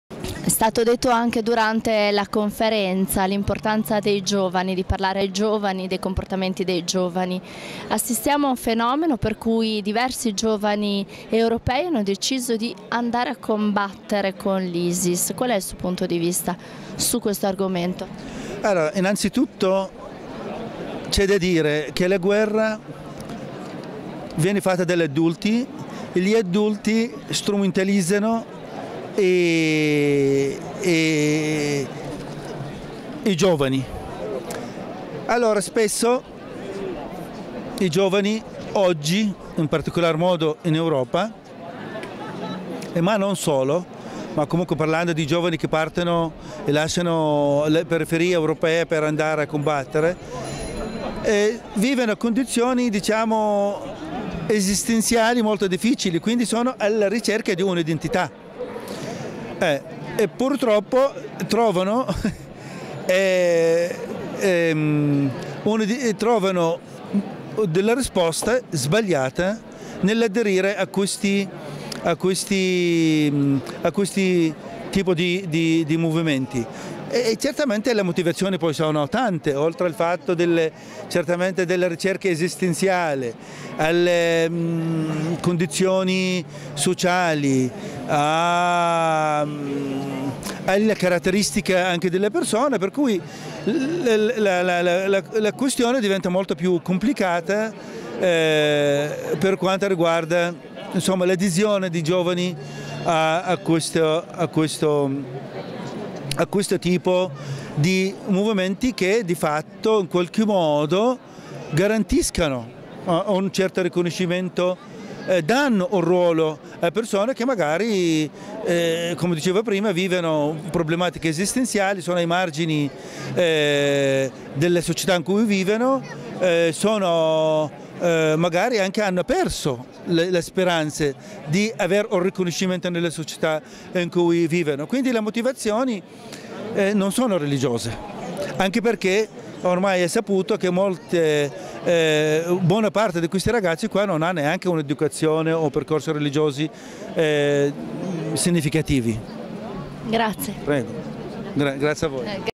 Intervista ad Adel Jabbar, "Pace tra le religioni: solo un'utopia?"
Intervista ad Adel Jabbar, sociologo, in occasione dell'ottava edizione del Festival Francescano "per forza o perdono". Riprese di Mauro Terzi, intervista a cura di Chiara Vecchio Nepita